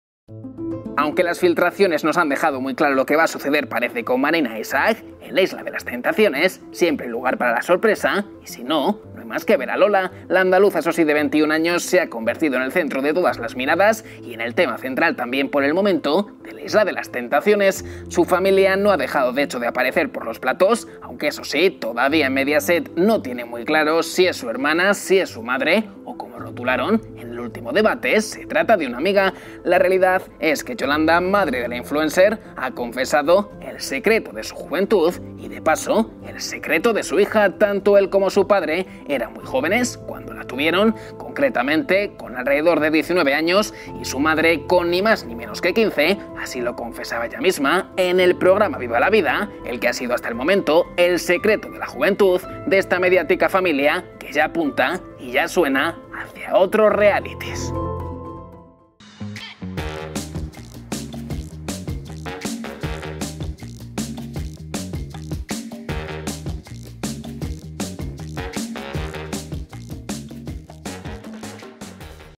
¡Qué fuerte! El secreto de Marina de La isla de las tentaciones ¡A la luz lo que todos se preguntan!
Ha sido, sin duda, uno de los temas más comentados tras el vídeo de Marina e Isaac. Y es que la concursante de La isla de las tentaciones ha centrado tanto todas las miradas que sus padres están también ahora en el punto de mira.